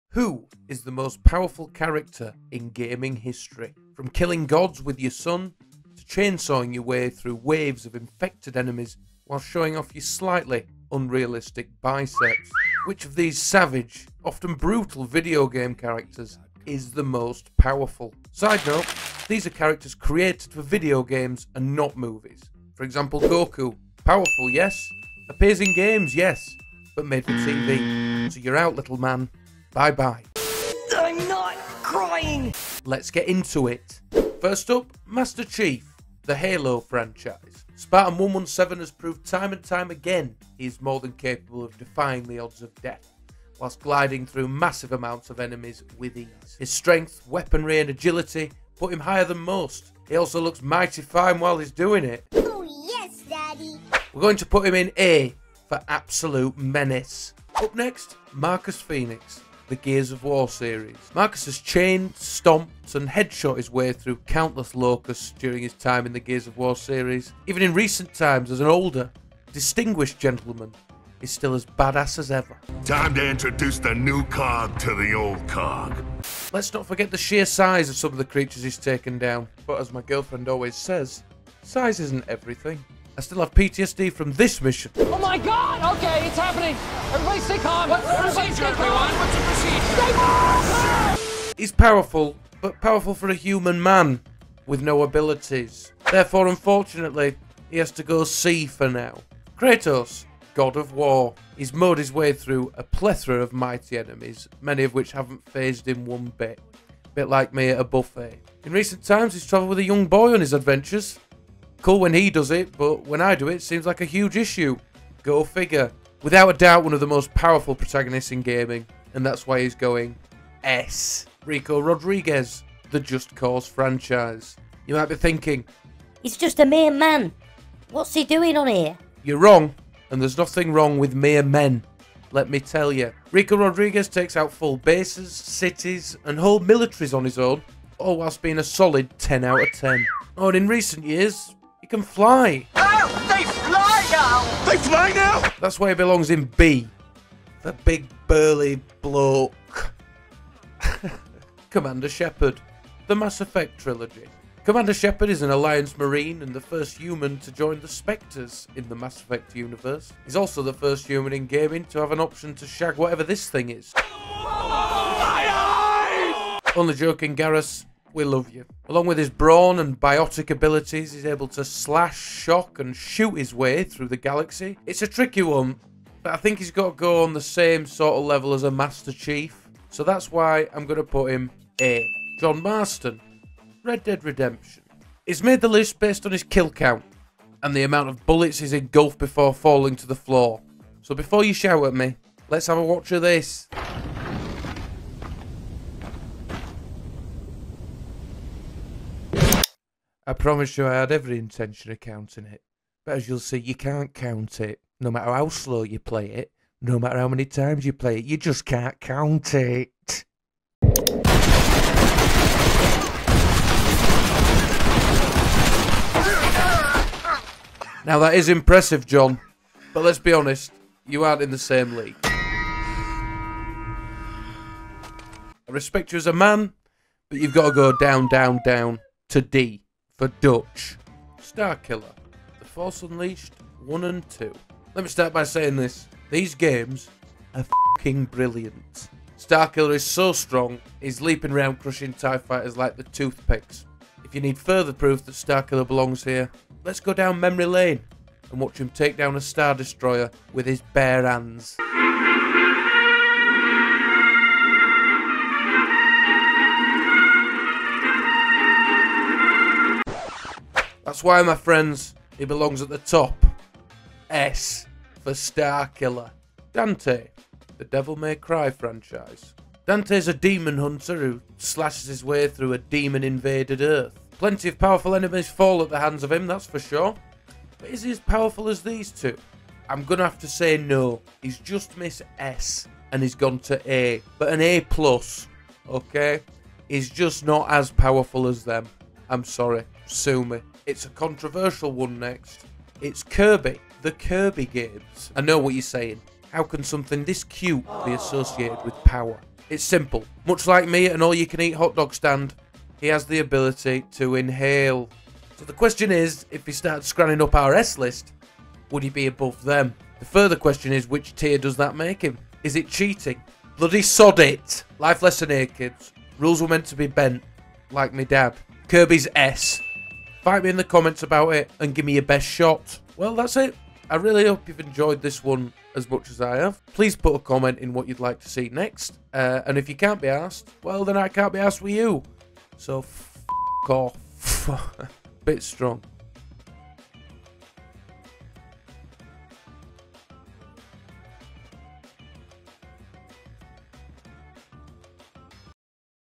Ranking The MOST Powerful Video Game Characters
In this video, we will be Ranking the MOST Powerful Characters in Video Game History.

From Kratos (God of War) to John Marston (Red Dead Redemption) and everyone in between. Prepare to see your favourite video game characters ranked.

This Tier List does not include TV or Movie characters who have appeared in games. So, in other words, Goku (Dragon Ball Z) will not appear.

Share what you think I should do next in the comments and some of your favourites that might have missed the list.

0:00 Intro
0:35 Master Chief
1:00 Marcus Fenix
1:40 Kratos
2:01 Rico Rodriguez
2:31 Commander Shepherd
3:03 John Marston
3:53 Starkiller
4:28 Dante
4:53 Kirby
5:24 Outro